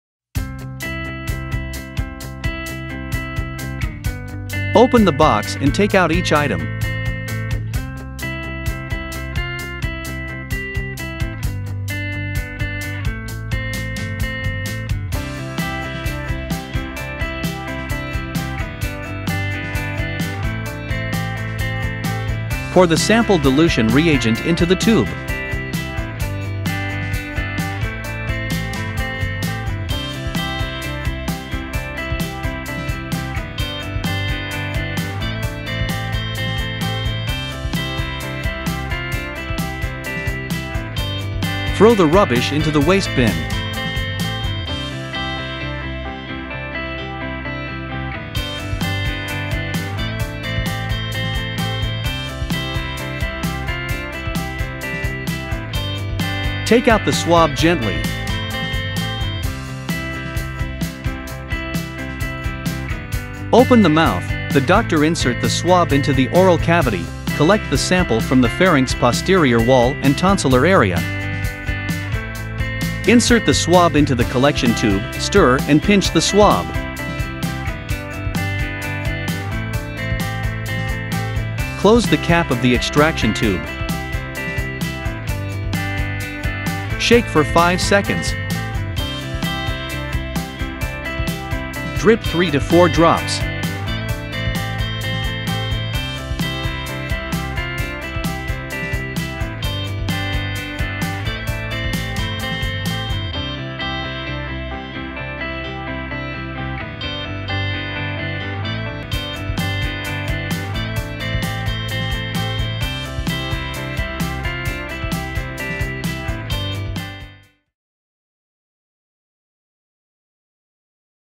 LONGSEE 2019-nCoV Ag Rapid Detection Kit by oropharyngeal swabs samples.
The kit is used to qualitatively detect the 2019-nCoV antigens in in-vitro samples such as , oropharyngeal swabs samples etc. The kit is only used to in-vitro auxiliary diagnosis.